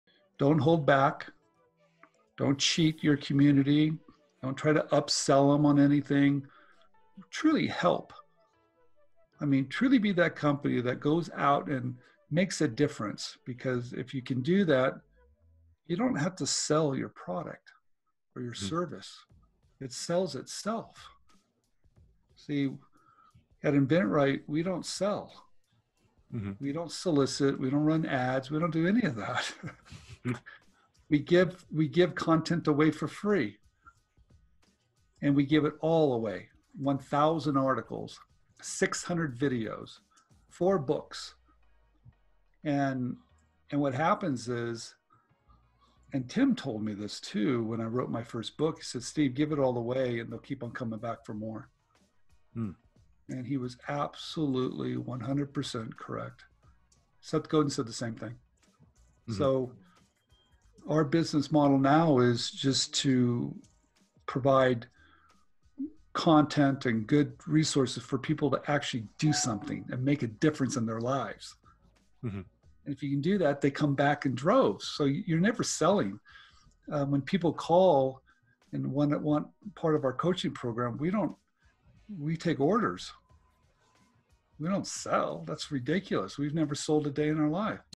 Stephen Key on giving away your content so you're never selling - Interview about 4-Hour Workweek
This is part of an interview with Stephen Key about his thoughts and contributions to the book The 4-Hour Workweek by Tim Ferriss.